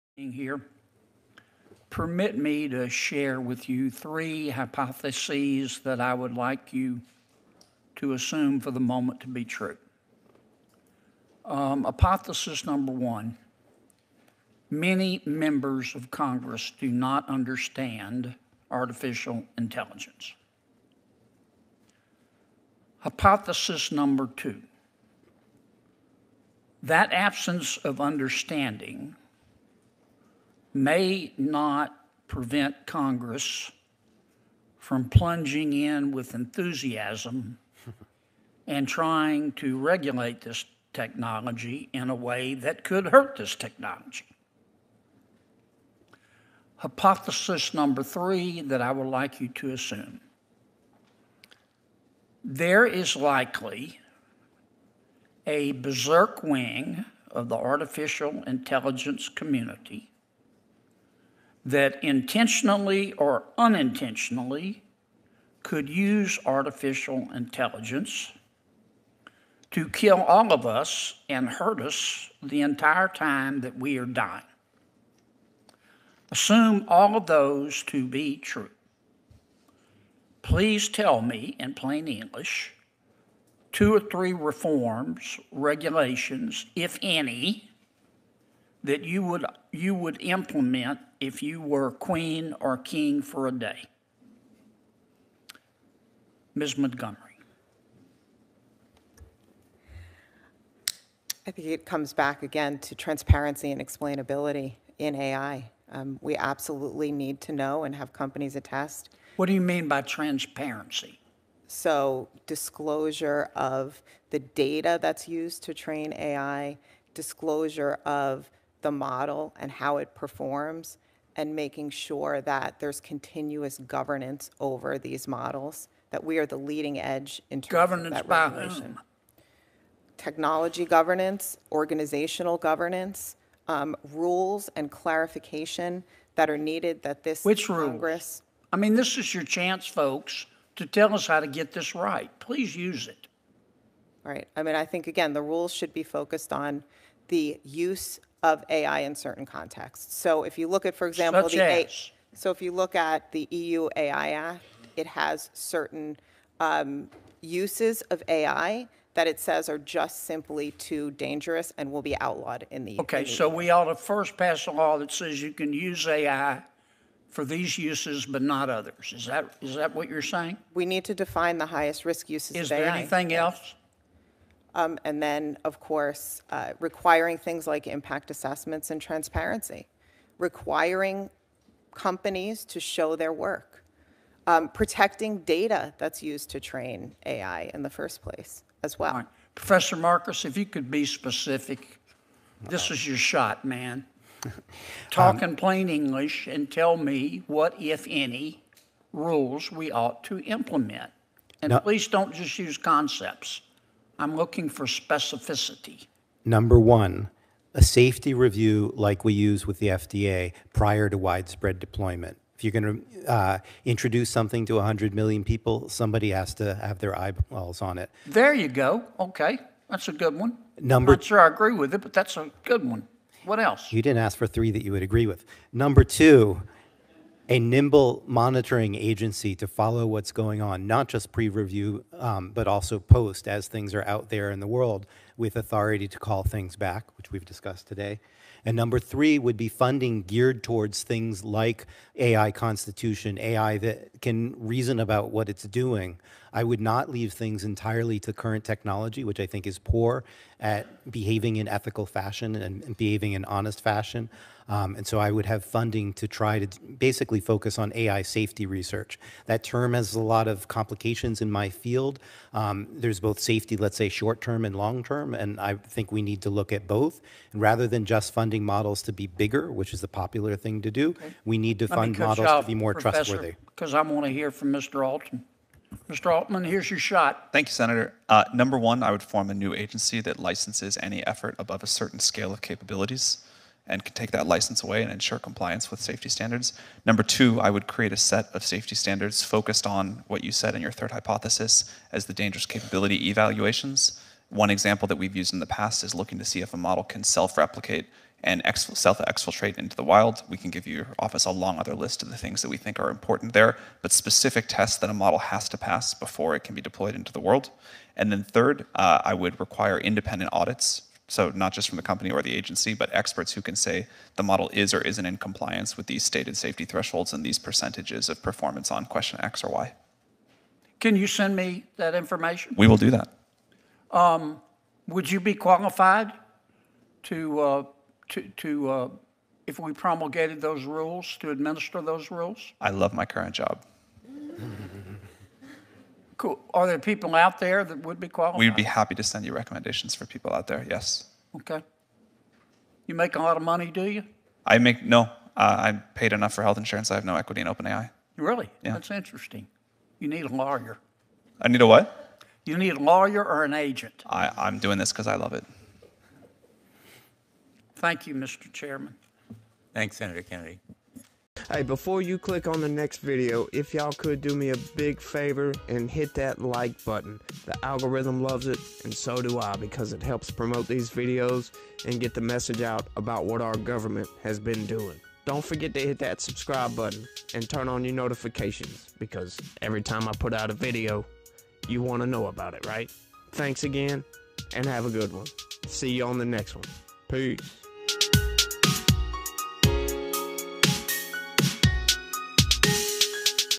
Senator Kennedy questions A.I. experts
Kennedy questions a.i. experts about  emerging technologies and how a.i. should be regulated